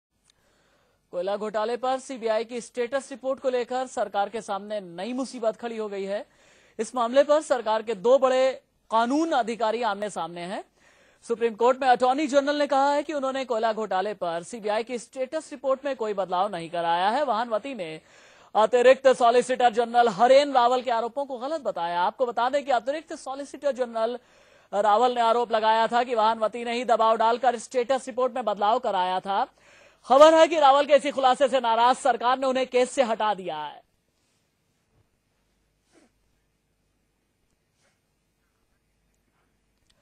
Coalgate: ASG Raval blames Vahanvati for interference
India News: Additional Solicitor General Harin Raval put the government in a spot after he accused Attorney General GE Vahanvati of trying to influence the CBI probe into the controversial allocation of coal blocks. Rawal wrote a letter to Vahanvati in which he made serious allegations against the Attorney General. A copy of the letter was marked to Law Minister Ashwani Kumar, who is at the centre of the controversy.